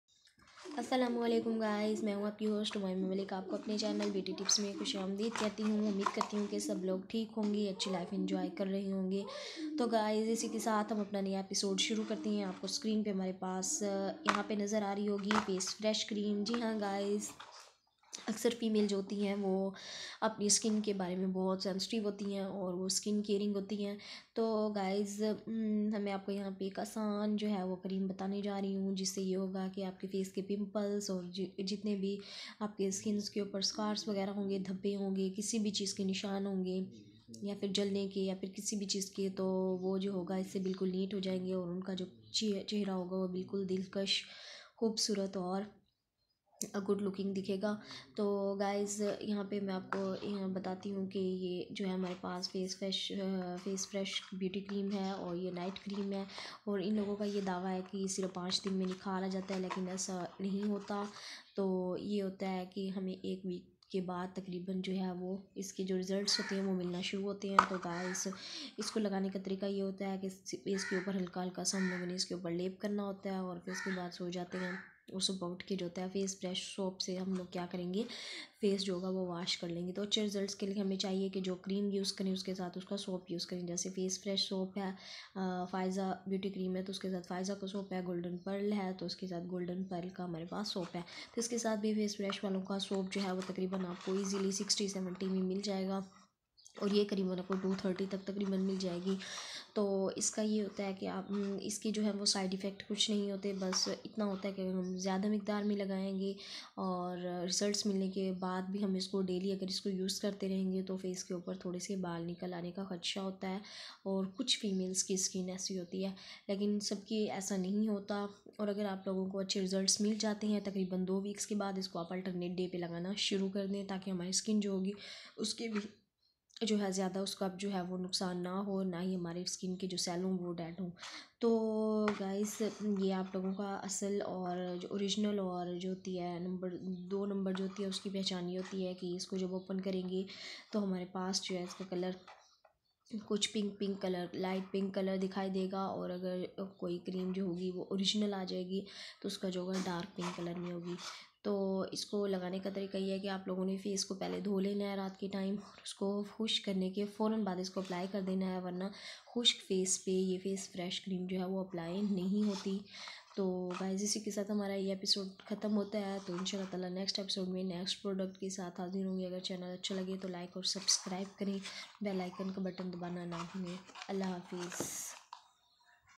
face fresh night cream is the best night cream|| gives results in seven days
many female face the problem of pimples and aceny
now face fresh cream that is night cream is used to solves this problem

best cream#night cream